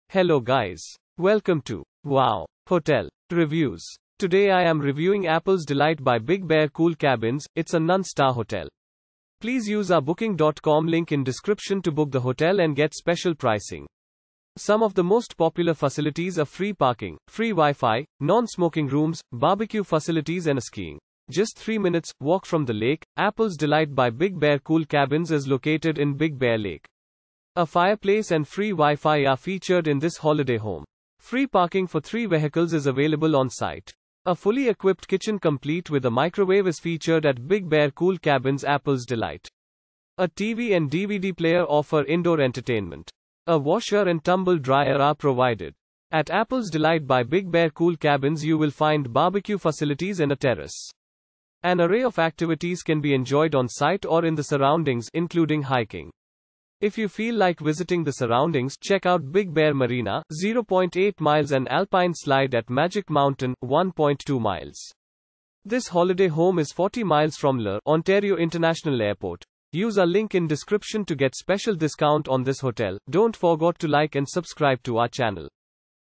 Apples Delight by Big Bear Cool Cabins Review - Big Bear Lake , United States of America
Apples Delight by Big Bear Cool Cabins

Just 3 minutes’ walk from the lake, Apples Delight by Big Bear Cool Cabins is located in Big Bear Lake. A fireplace and free WiFi are featured in this holiday home. Free parking for 3 vehicles is available on site.
A fully-equipped kitchen complete with a microwave is featured at Big Bear Cool Cabins Apples Delight. A TV and DVD player offer indoor entertainment. A washer and tumble dryer are provided.
At Apples Delight by Big Bear Cool Cabins you will find barbecue facilities and a terrace. An array of activities can be enjoyed on site or in the surroundings, including hiking.
If you feel like visiting the surroundings, check out Big Bear Marina (0.8 mi) and Alpine Slide at Magic Mountain (1.2 mi). This holiday home is 40 mi from LA/Ontario International Airport.